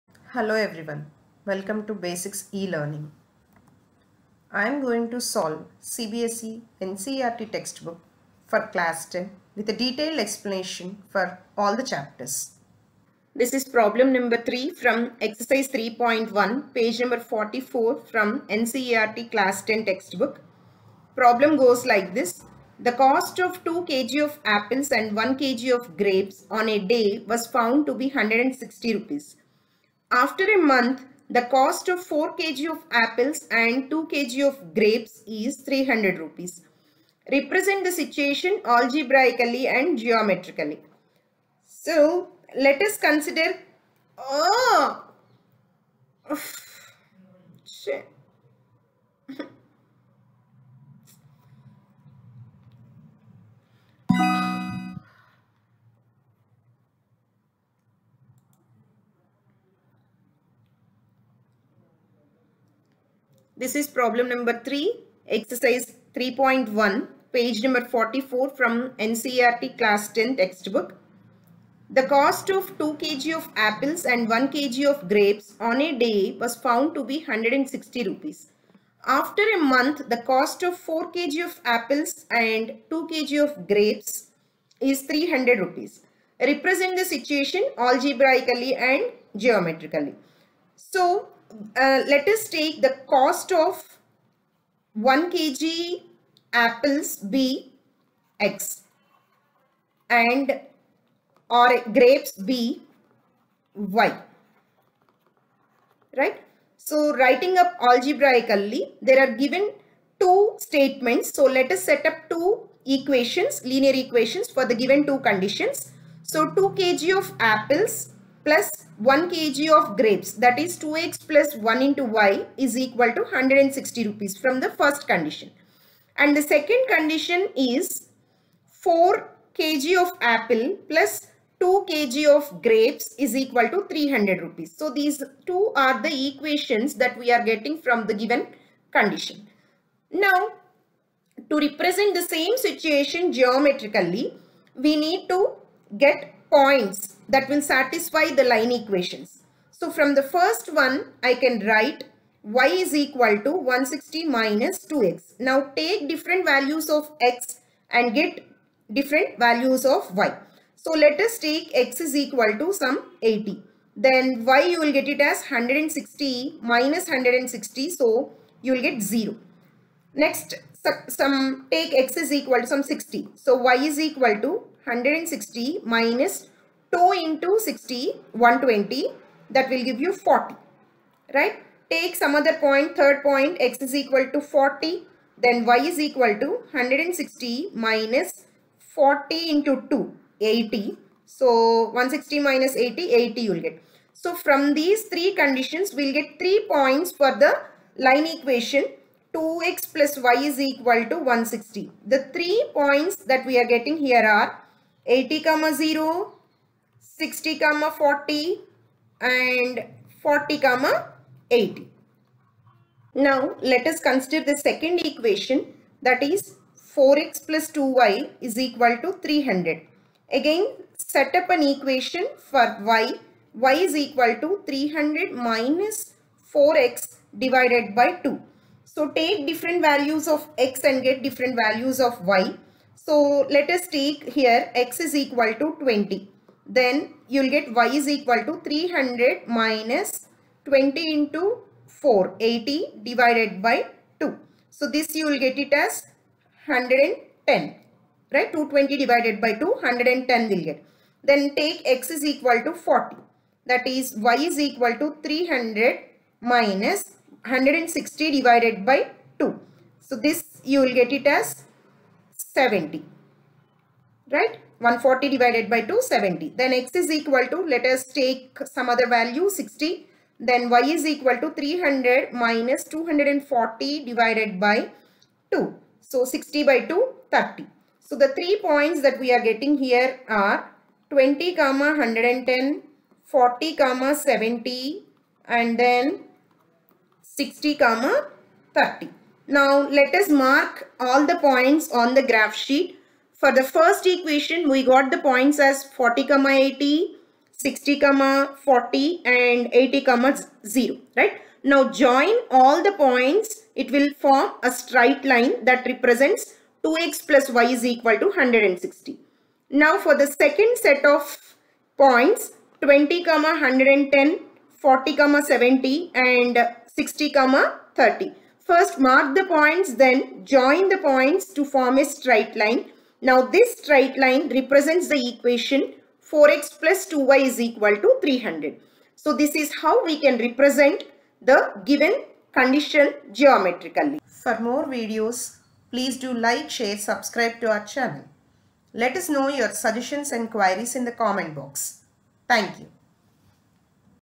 EX 3.1 | PROBLEM-3 SOLUTION | CBSE CLASS 10 | NCERT MATHS | CHAPTER 3
This video contains the solution for problem -3 Ex : 3.1 , NCERT textbook with a detailed explanation along with tips and tricks.